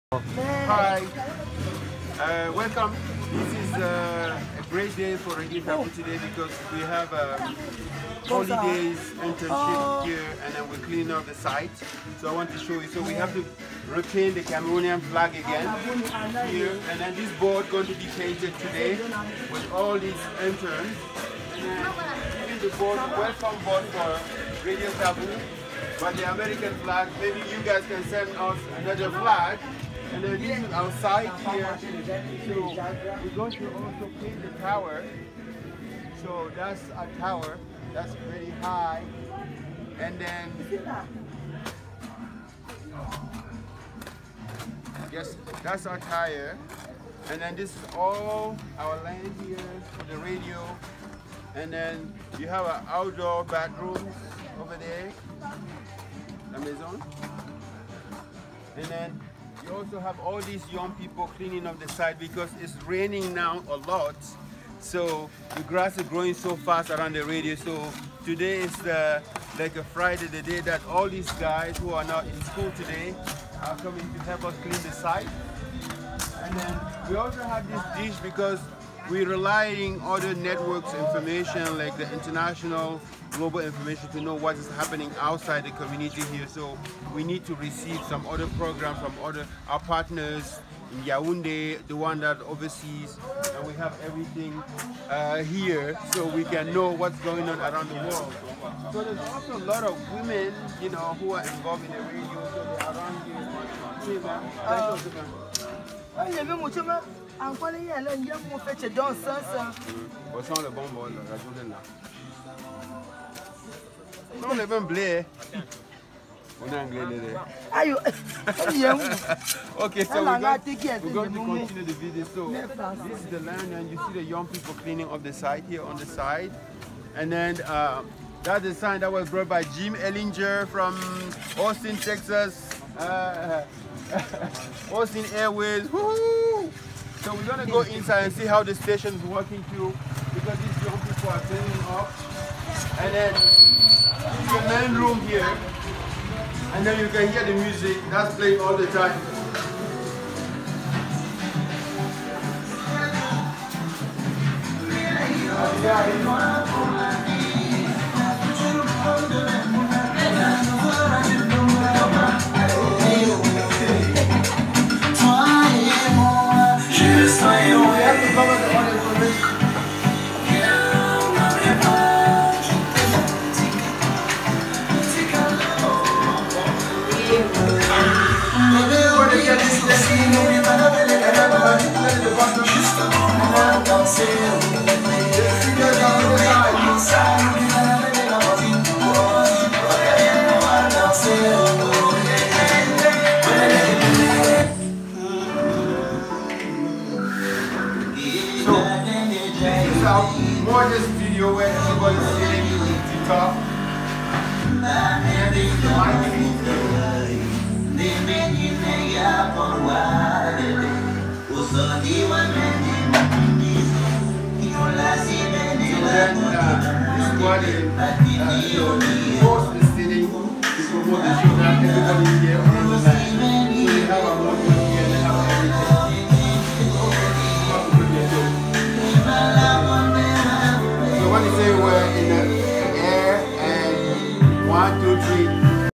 Radio Taboo (Solar Community Radio Station) in Cameroon.
Issa Nyaphaga, artiste and radio producer presents Radio Taboo, just one year after this off-grid development project was launched. Radio Taboo’s goal is to broadcast for 1 million people in 10 languages, spoken in the Center region of Cameroon. Most listeners are underserved and minority groups; predominantly indigenous.
In Cameroon, community radio is the perfect media for the people. There are 25 million people in Cameroon and 262 languages, more spoken than written.